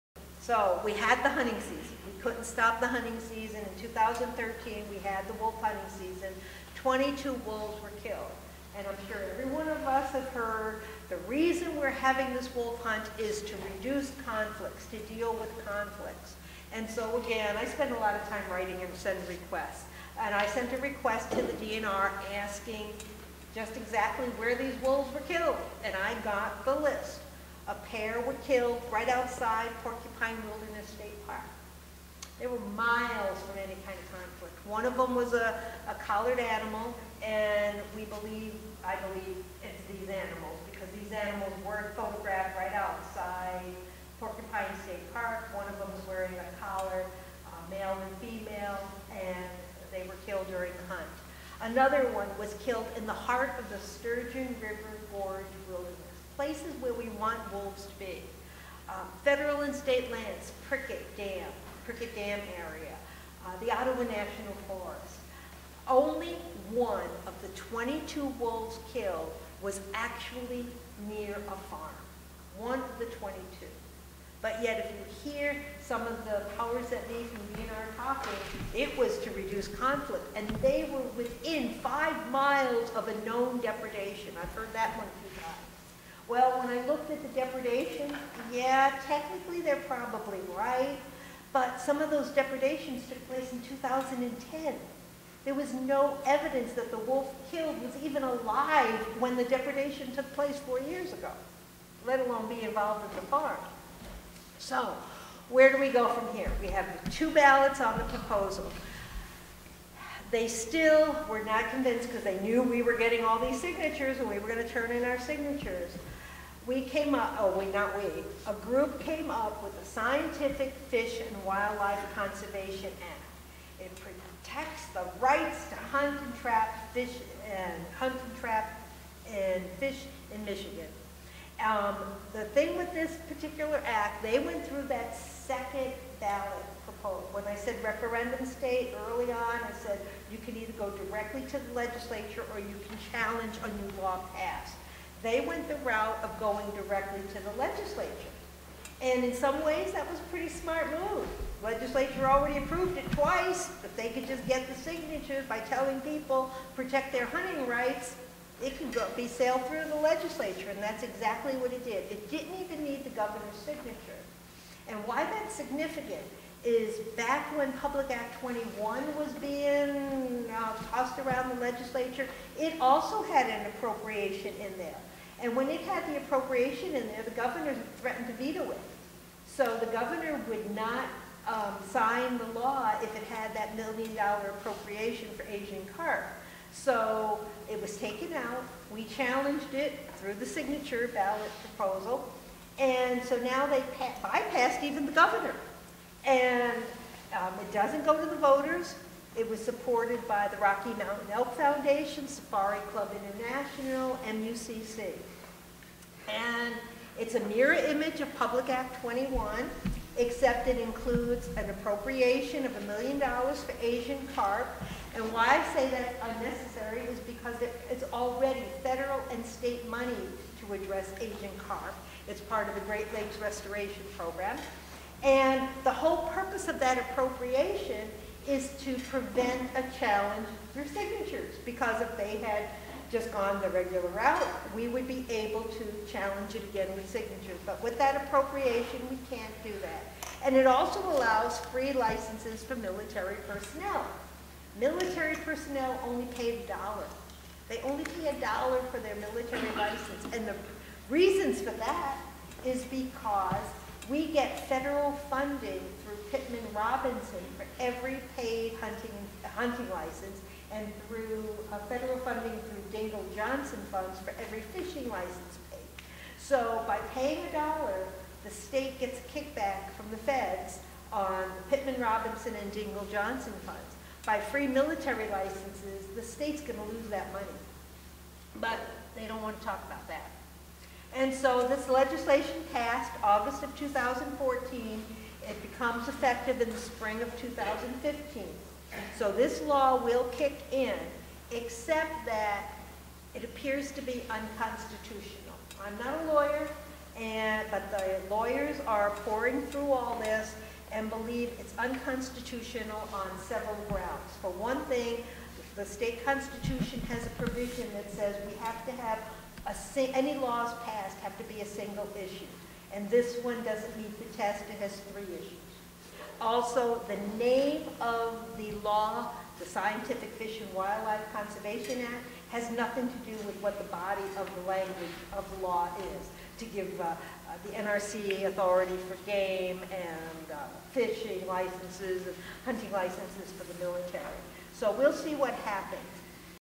nancy warren hancock6
Nancy Warren of Wolfwatchers talks about the 22 wolves killed during the 2013 wolf hunt in Michigan. Only one was ever near a farm and there was no evidence of any of them being involved in depredations. She also explains how a group of pro-wolf-hunt groups introduced the Scientific Fish and Wildlife Conservation Act, which did not require the Governor's signature and cannot go to the voters for referendum because of an appropriation attached. She also says it may be unconstitutional. (Video by Allan Baker for Keweenaw Now)